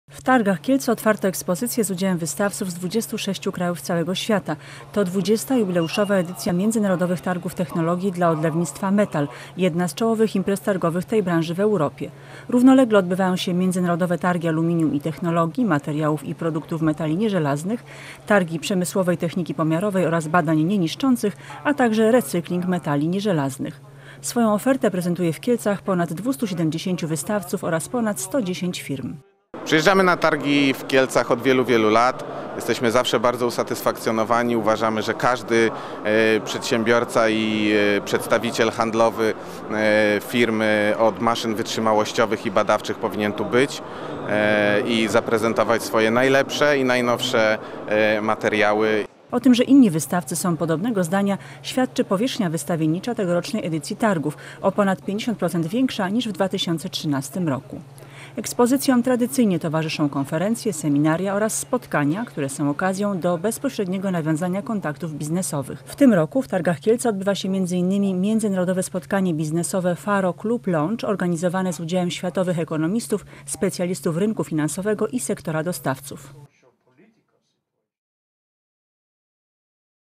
Jubileuszowy METAL w Targach Kielce - ITV Kielce
W Targach Kielce otwarto ekspozycje  z udziałem wystawców z 26 krajów całego świata. To 20-ta, jubileuszowa edycja Międzynarodowych Targów Technologii dla Odlewnictwa METAL - jedna z czołowych imprez targowych tej branży w Europie. Równolegle odbywają się  Międzynarodowe Targi Aluminium i Technologii, Materiałów i Produktów Metali Nieżelaznych, Targi Przemysłowej Techniki Pomiarowej oraz Badań Nieniszczących, a także Recykling Metali Nieżelaznych